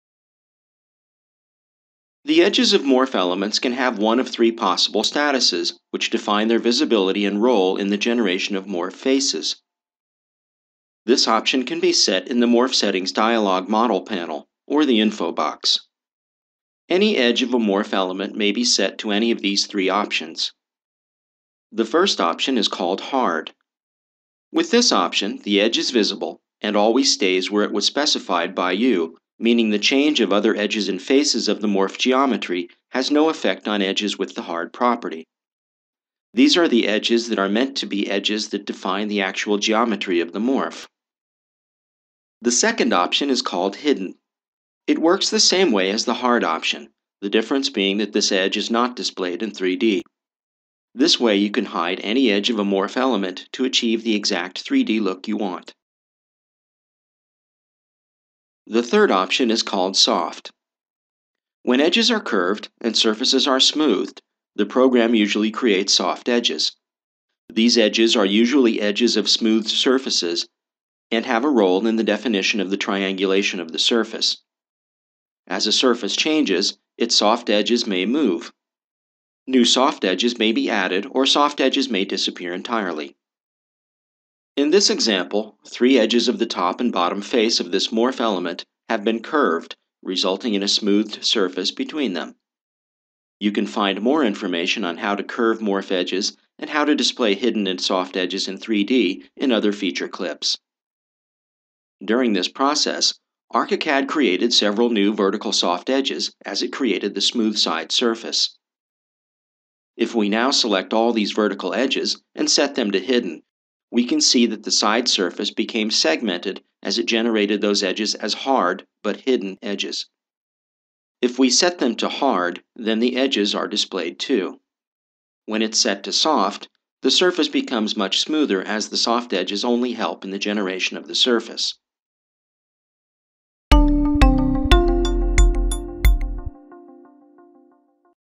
The ARCHICAD MORPH Tool - Edge Generation Method of MORPHs
ArchiCAD 16 New Features - The MORPH Tool
Edge Generation Method of MORPHs

ArchiCAD 16 introduces direct modeling capabilities into the native BIM environment with its brand new MORPH™ tool.

The edges of MORPH elements can have one of three possible statuses, which define their visibility and role in the generation of MORPH faces.

Title image:
Vennesla Library & Cultural Center, Norway